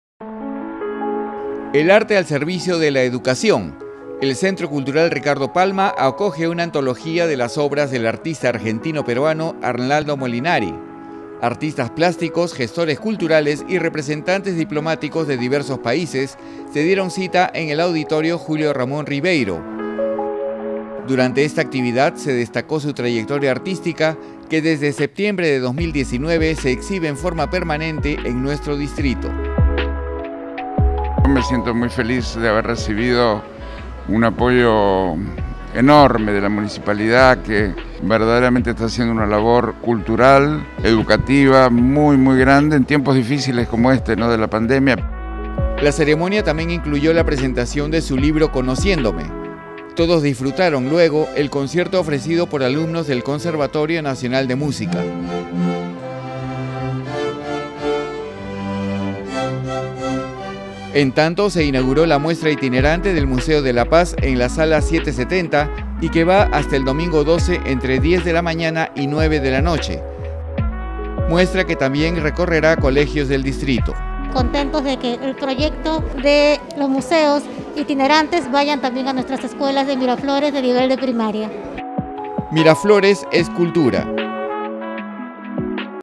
MUSEO DE LA PAZ
El Centro Cultural Ricardo Palma acoge una antología de las obras del artista argentino-peruano Arnaldo Molinari.